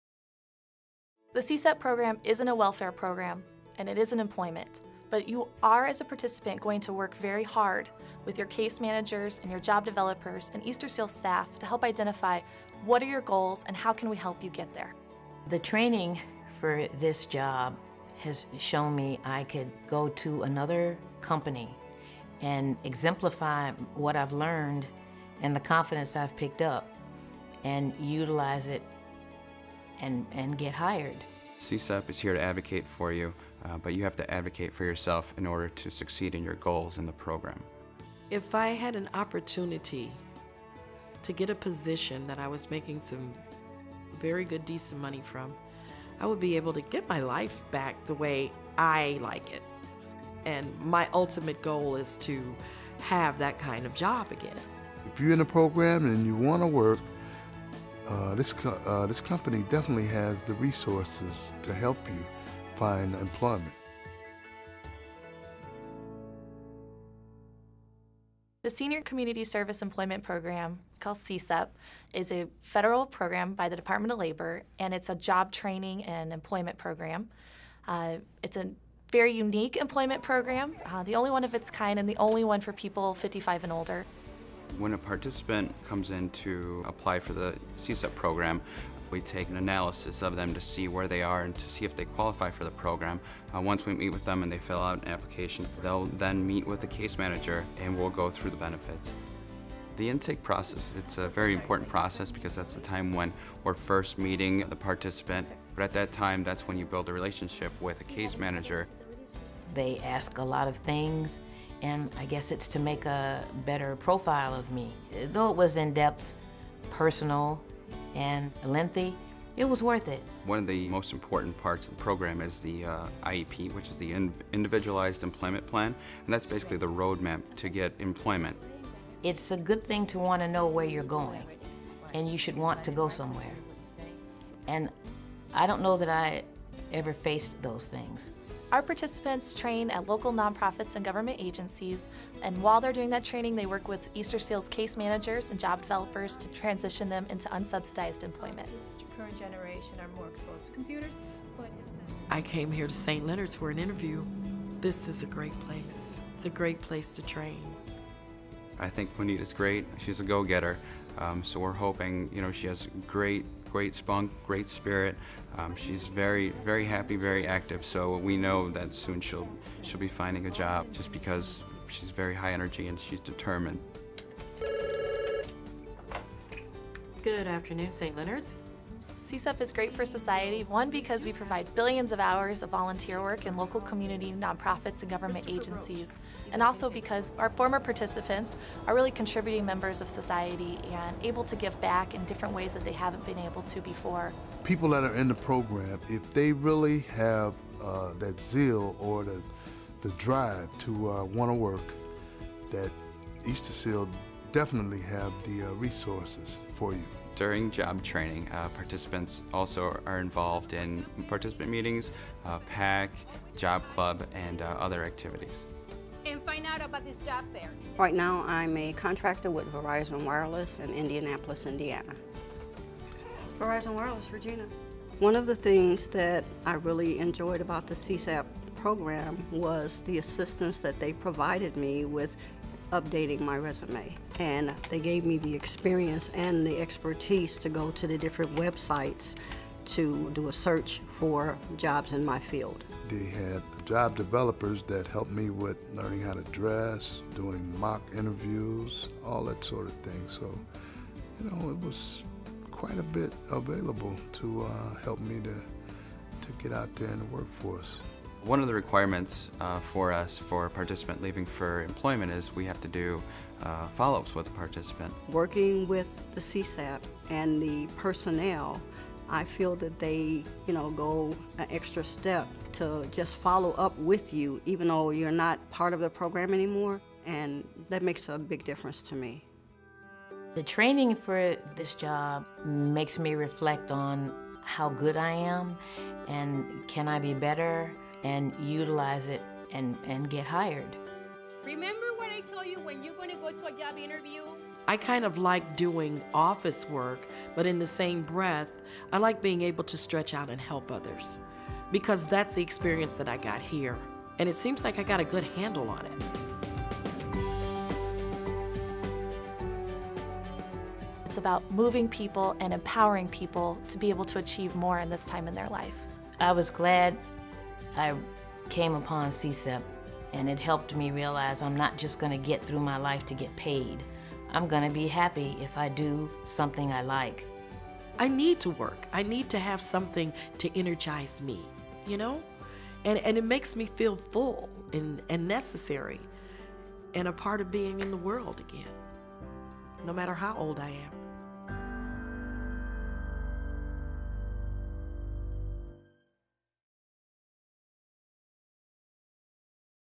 Make Your Experience Count
Learn about what to expect out of the Senior Community Service Employment Program (SCSEP) with Easter Seals, meet some current and former SCSEP job seekers, and hear first-hand how to make this program work for you.